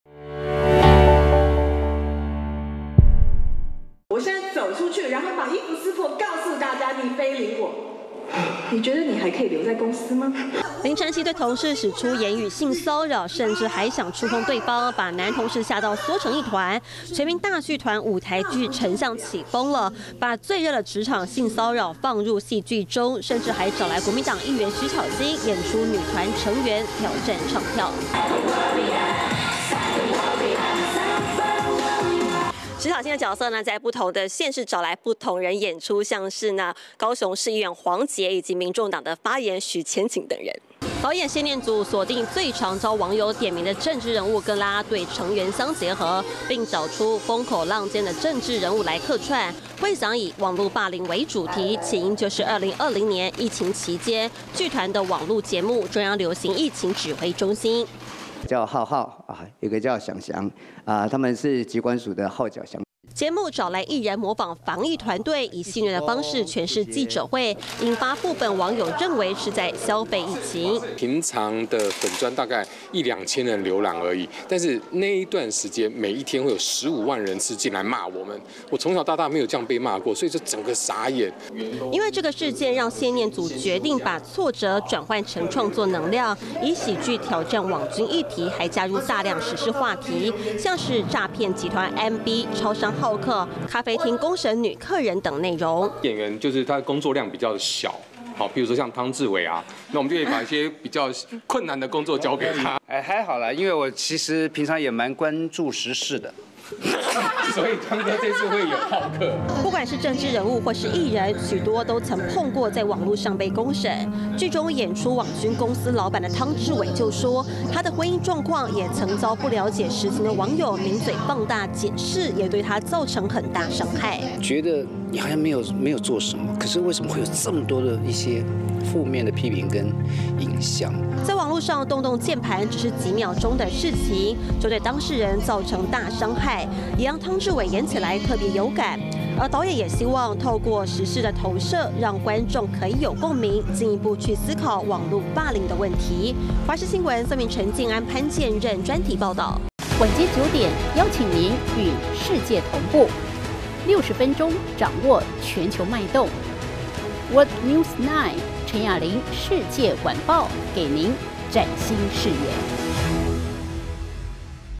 網路霸凌為題材！ 舞台劇納入最新時事｜華視新聞 20230609
除了最近備受爭議的性騷擾事件延燒，過去網路霸凌事件，也時有所聞，全民大劇團就以網路霸凌、網軍為題材，以喜劇的方式創作舞台劇「丞相，起風了」！導演謝念祖認為，檯面上最常受網軍攻擊的，就屬政治人物跟啦啦隊成員，因此找來藍綠白陣營的政治人物、演出女團成員，藉此來探討網路霸凌事件，同時也加入時事議題，希望讓戲迷看了更有感！